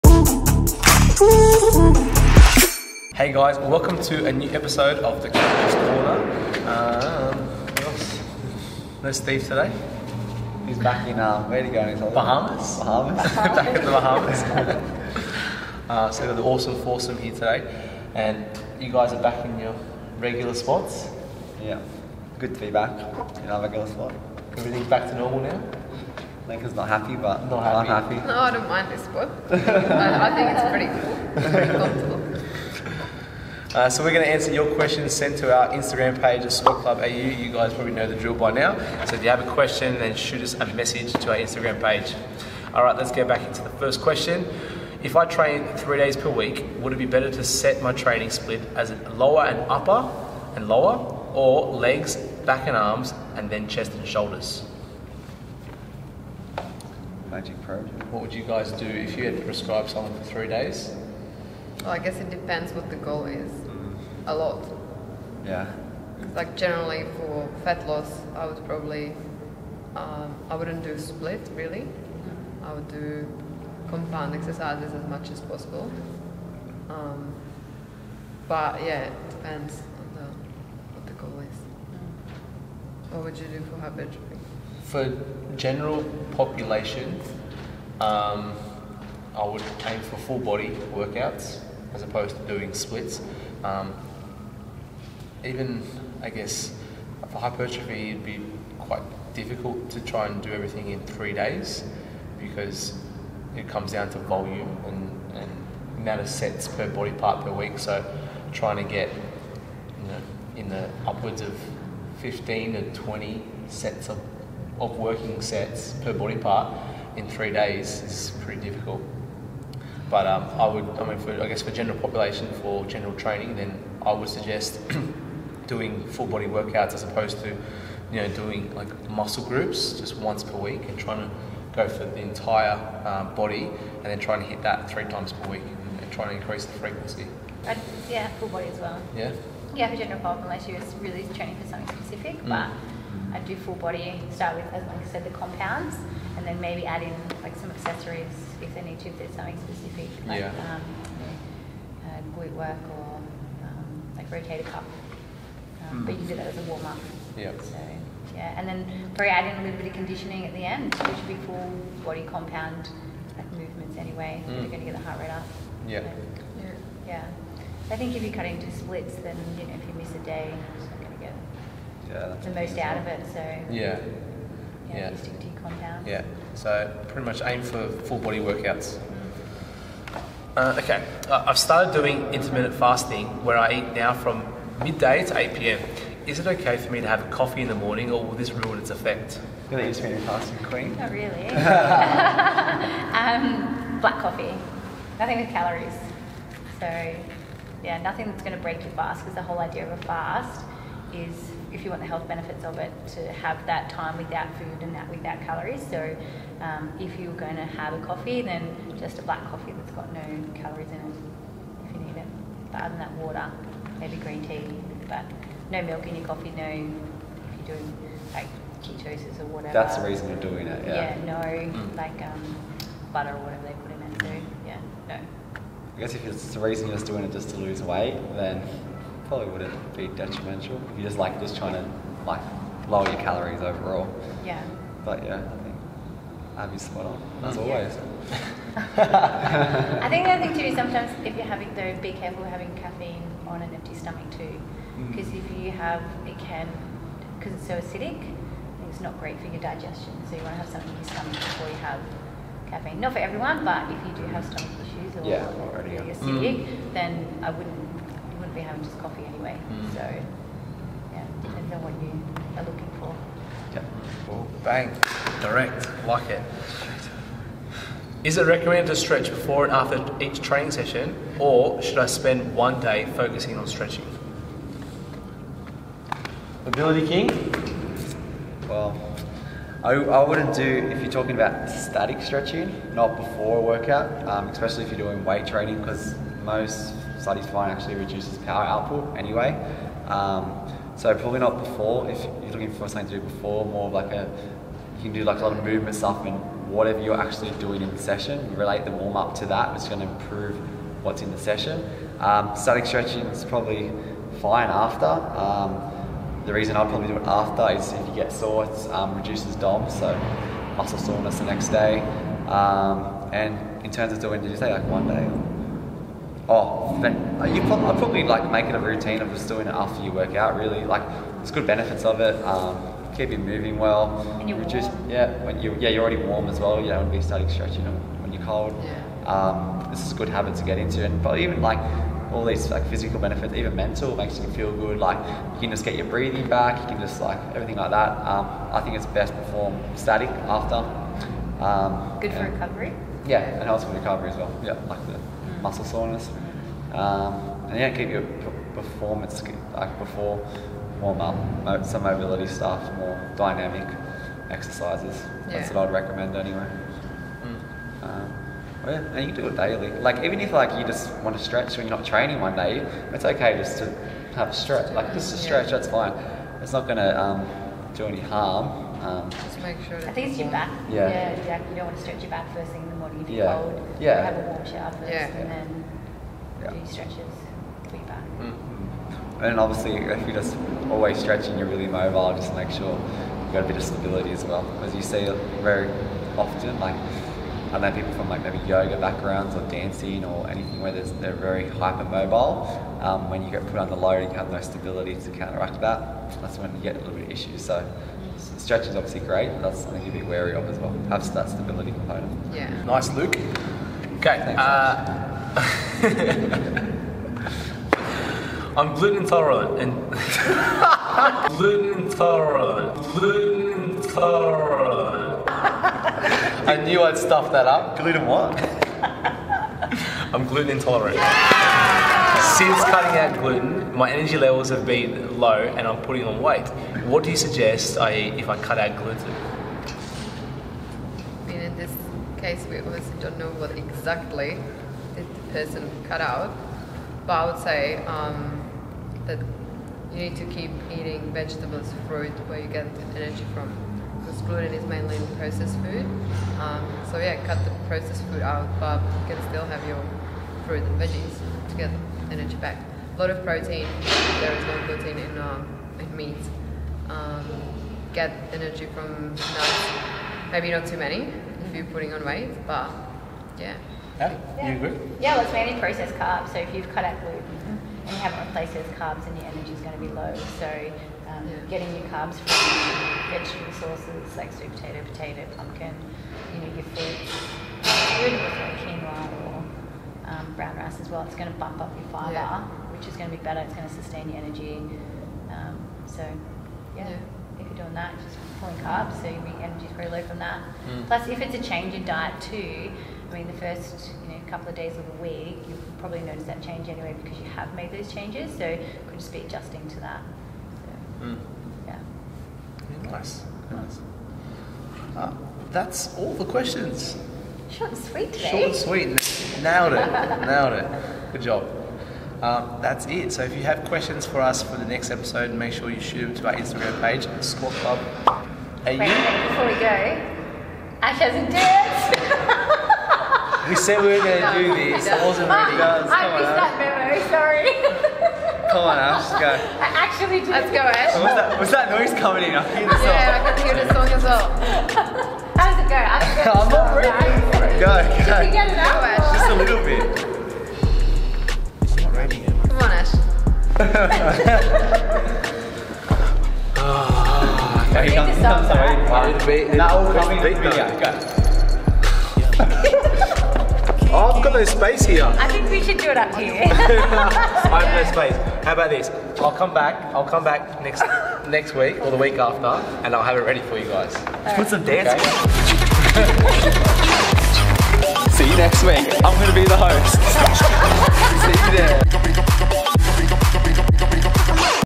The BEST three day split for your training || Coaches Corner
We dive into our next episode of Coaches Corner to answer your questions sent into our instagram page - @squatclubau

LOCATION:
Squat Club
1/41 Dunn Rd,
Smeaton Grange,